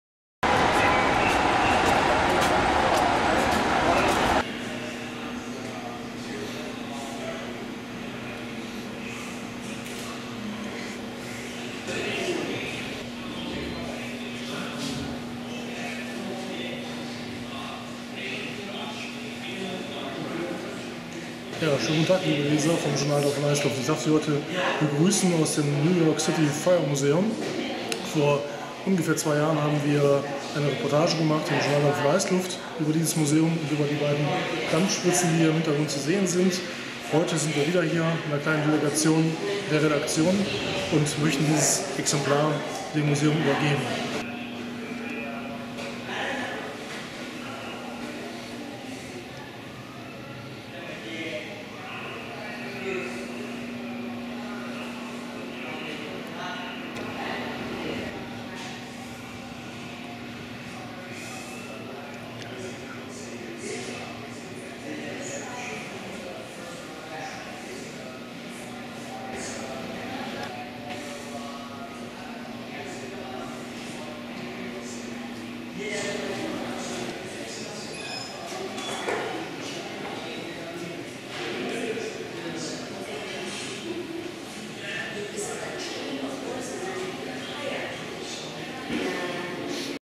New York City Fire Museum - Journal Dampf & Heißluft
Die Redaktion vom Journal Dampf & Heißluft (Neckar-Verlag) überreichte im September 2014 dem New York City Fire Museum ein Belegexemplar der Sonderausgabe "Dampf auf Tour 2". Zu sehen sind in diesem Video unter anderem die beiden im Museum ausgestellten Dampffeuerspritzen.